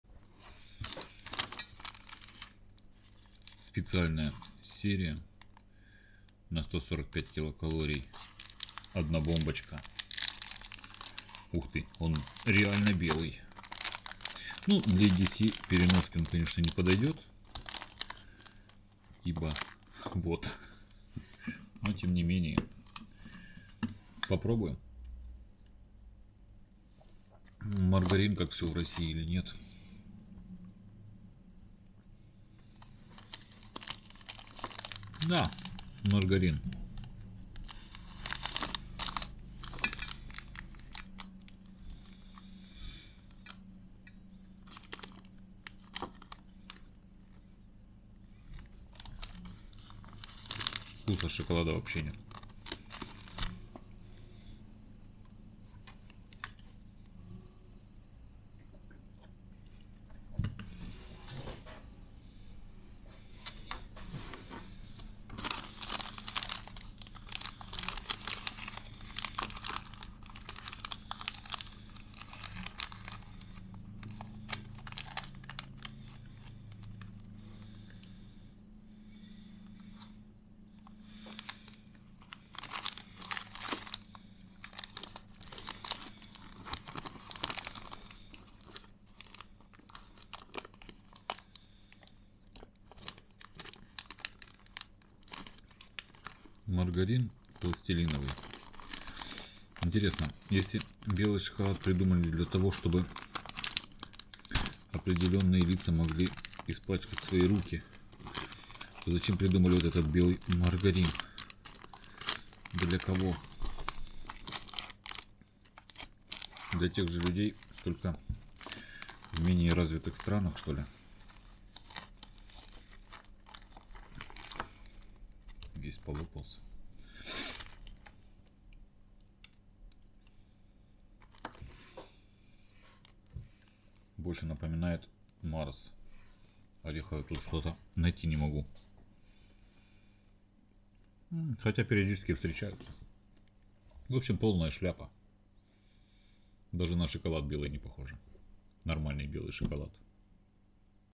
Батончик Nestle "Nuts limited edition" белый
Nuts начал меняться! ♥ Самые упорные консерваторы в мире шоколада выпустили новинку - Nuts Limited Edition в белом шоколаде! ♥ Есть ли разница, кроме цвета шоколада?
Это тот вид шоколада, который просто так не растает во рту, его придется пожевать. Классно, что внутри присутствует цельный фундук! Он гораздо полезнее того же арахиса из Сникерса. Вообще вкус Nuts знаком с детства - орехово-ванильно-карамельный!

Белый шоколад хоть и придал вкус белого шоколада, но всё же не могу сказать, что он кардинально изменил вкусовое восприятие шоколадки. Это всё тот же старый добрый, знакомый с детства Nuts.

Отлично сочетается с кофе и чаем!